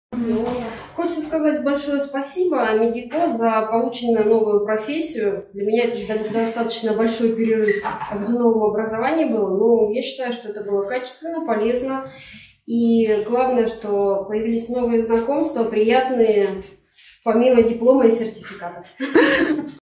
Отзыв студента
Студент рассказывает о институте МИДиПО по окончании курсов "Медицинский массаж"